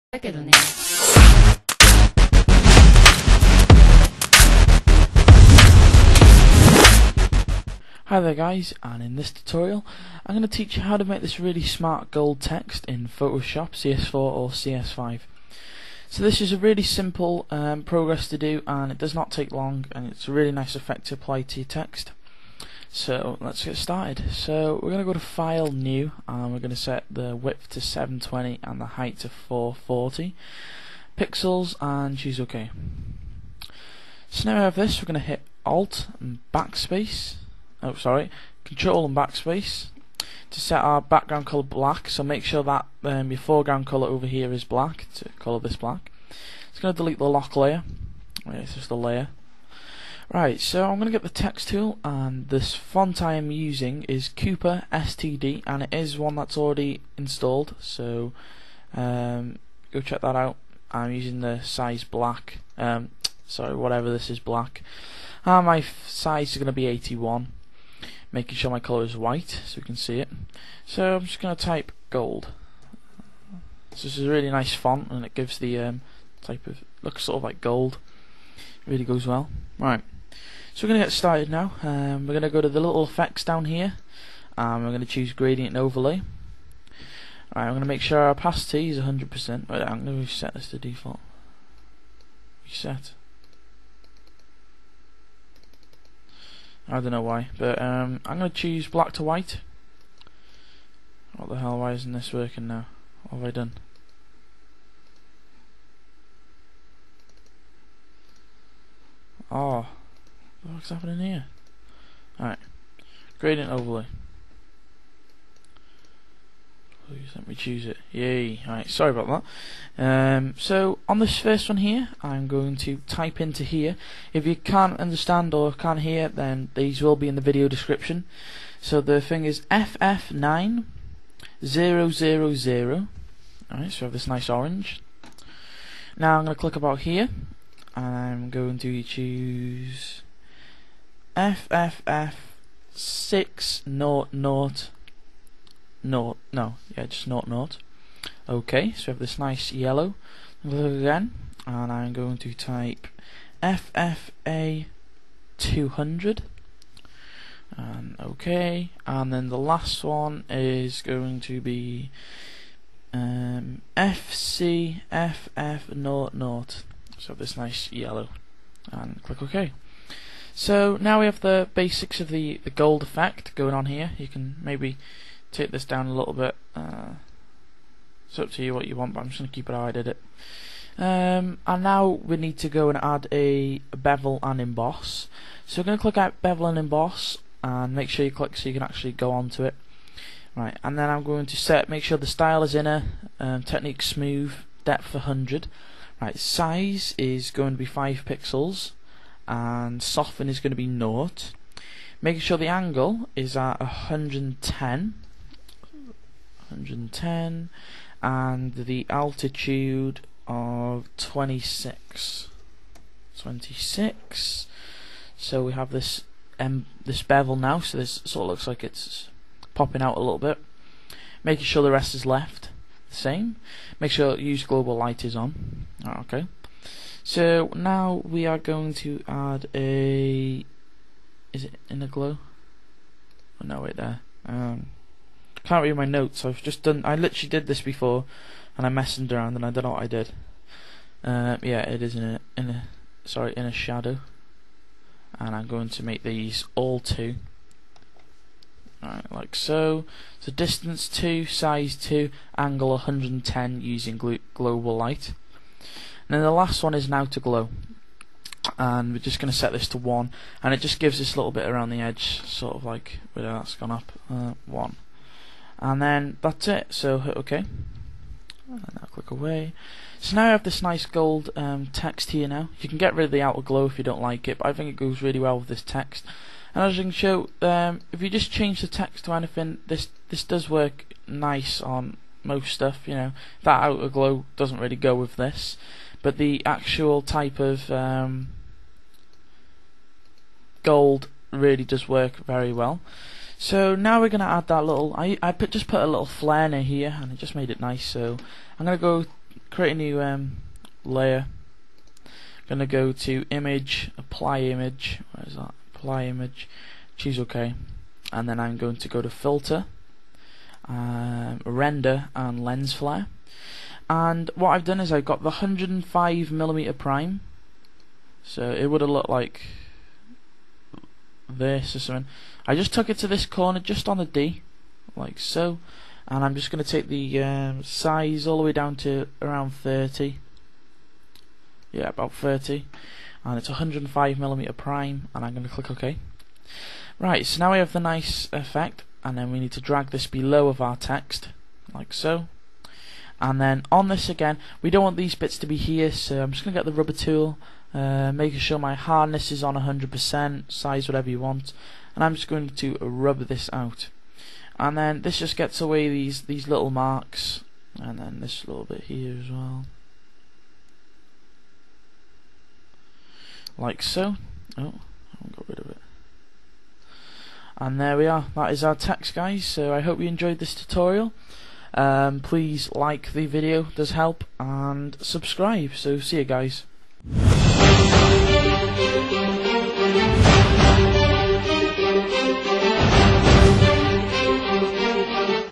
Photoshop cs4 or cs5 Tutorial: Create Shiny Gold Text
This Tutorial shows you how to make ace gold text using adobe photoshop cs4 or cs5.

colours for gradient overlay =
left to right
ff9000 - fff600 - ffa200 - fcff00